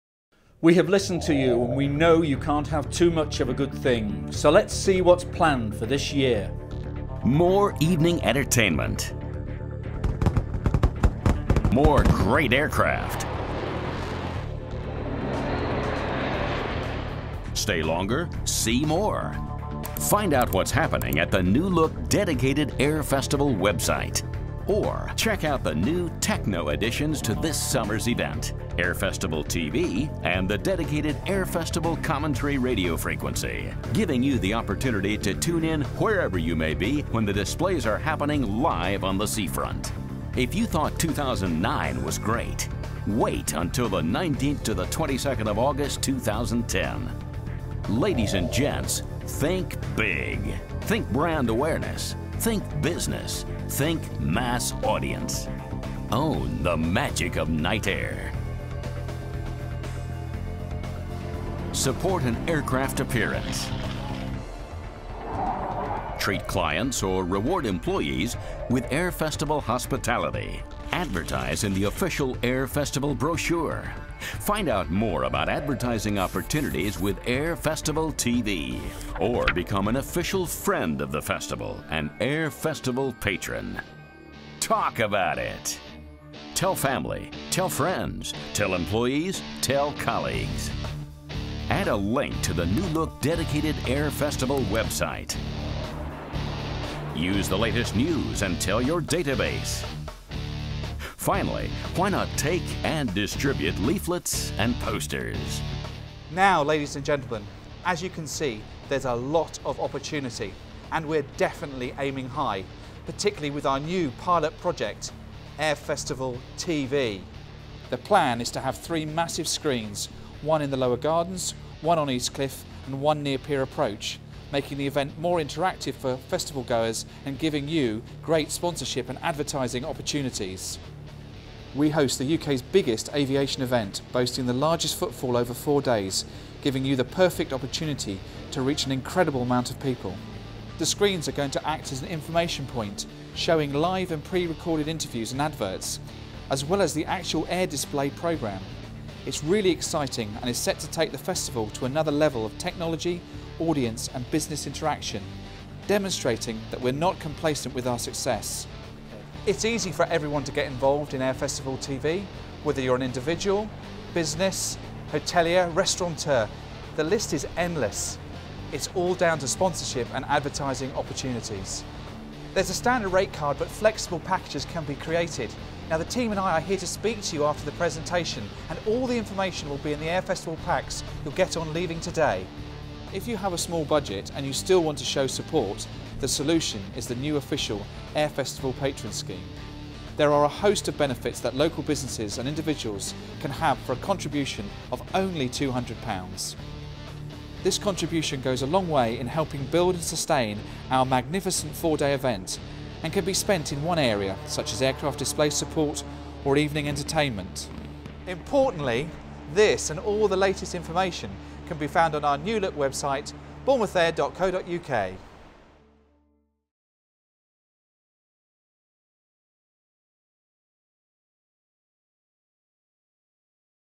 Get Involved with the Bournemouth Air Festival 2010
All the latest info on the exciting new editions to this year's festival line-up, as well as promising sponsorship opportunities and all the ways you can get involved.